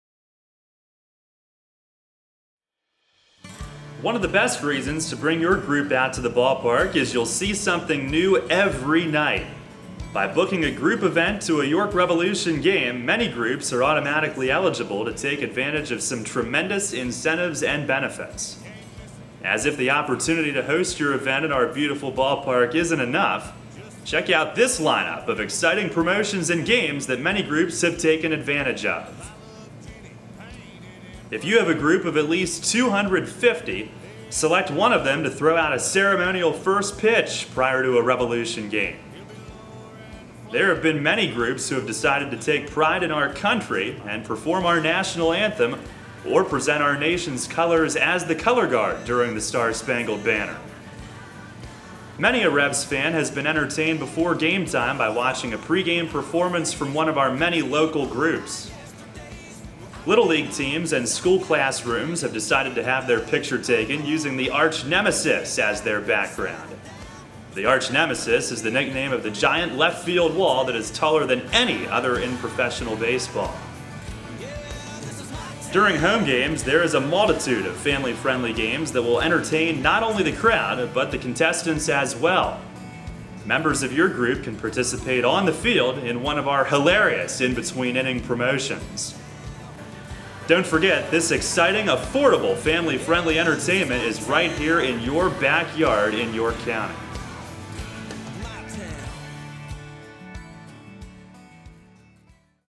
Group Events Tour INCENTIVES (why we come!)
Welcome to the York Revolution Group Events Tour!  We've gone digital, and you can now view our group event options online.  Instead of a multi-page booklet, the York Revolution have decided that great video, cool music, and some pretty pictures (hosted by Darrell Henry, the Voice of the Revolution) is the best way to show our fans what we're all about.  Enjoy the Group Events Tour and we hope to see you and your group at beautiful Sovereign Bank Stadium!